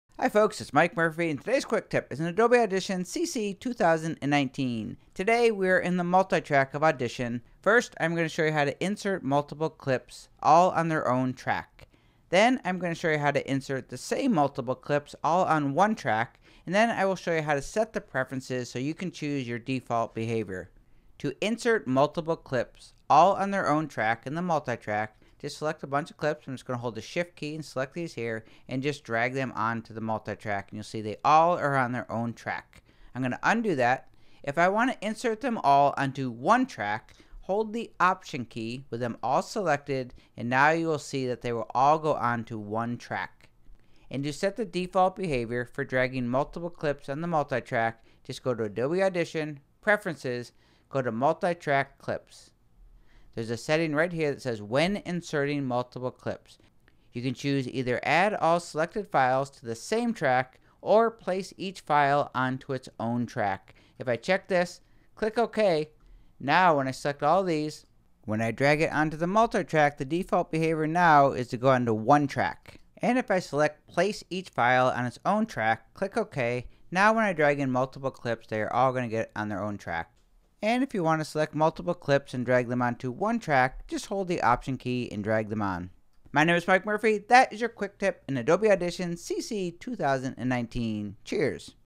How To Insert Multiple Clips in Multitrack of Adobe Audition CC 2019
This tutorial is in the Multitrack of Adobe Audition CC 2019
1. How to insert multiple clips each on their own track
2. How to insert multiple clips all on one track
3. How to set Preferences for inserting multiple clips

By default:
Dragging multiple clips on the multitrack will add clips to their own tracks
Hold the Option key to insert multiple clips all on one track

How to insert multiple clips each on their own track
1. Open Multitrack Adobe Audition
2. Import Audio Files/Clips
3. Select multiple clips (Shift + Click)
4. Drag onto multitrack and each clip will be on its own track

How to insert multiple clips all on one track
1. Open Multitrack Adobe Audition
2. Import Audio Files/Clips
3. Select multiple clips (Shift + Click)
4. Hold the Option Key and drag onto Multitrack
5. Each clip will line up next to each other on one track

How to set Preferences for inserting multiple clips

1. Open Multitrack Adobe Audition
2. Click Adobe Audition in Menu Bar
3. Click on Preferences
4. Click on Multitrack Clips
5. Go to section ‘When Inserting Multiple Clips’
6. Click the radio button for which you want to be the default setting
    1. ‘Add all selected files to the same track’ or
    2. Place each file onto its own track